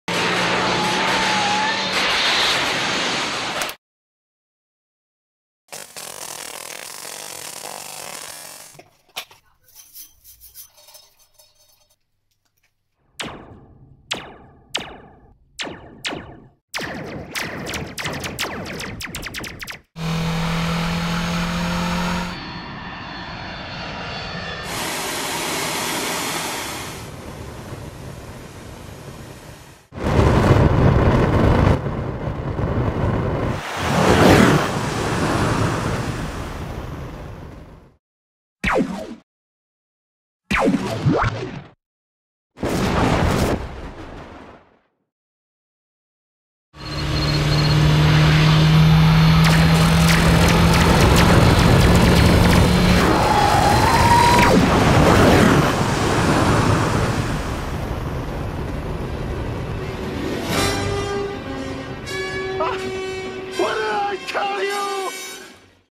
Short Sound Design - De Lorean Back to the Futur
#3 Here is a short video Marty ! Make it sound vintage !
Recreate the sound of the DeLorean using a slinky, a torch and some other things. 🔥📺🔊🎧